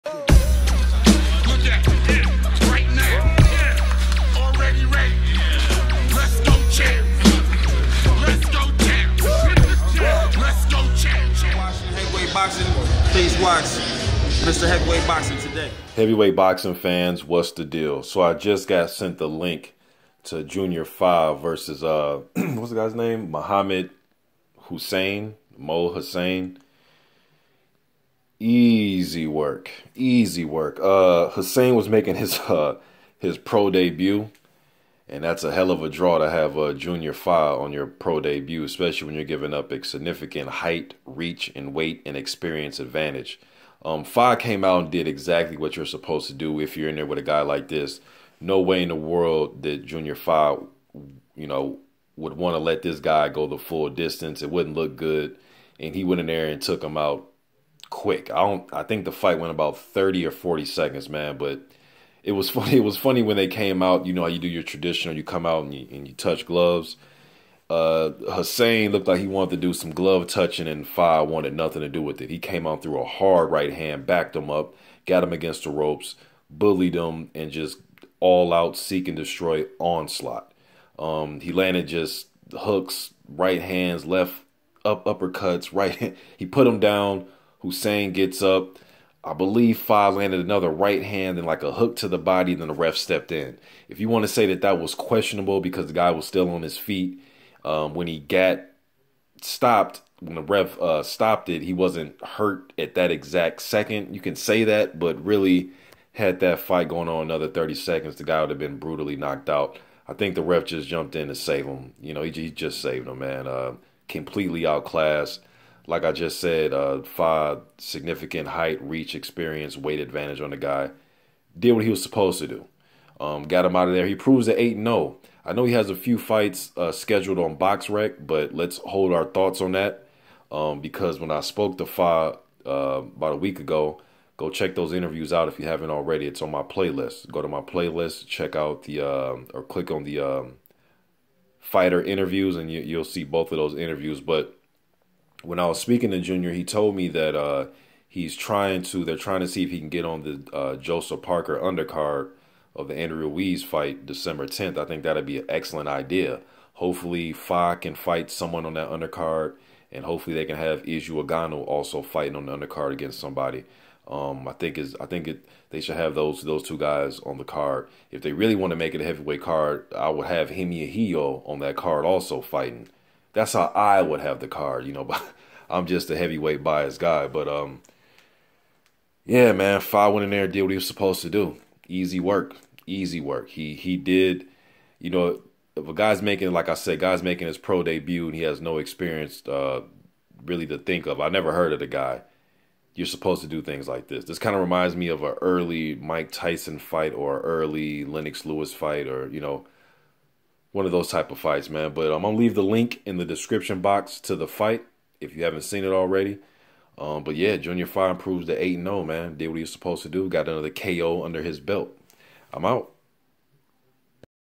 Junior Fa (Joseph Parker Rival) Improves To 8-0 My Thoughts On 1st Round TKO Victory!!
MrBOXINGTODAY2 SPEAKS ON RISING KIWI HEAVYWEIGHT PROSPECT JUNIOR FA 1ST ROUND TKO VICTORY OVER MOHAMMAD HUSSAIN!!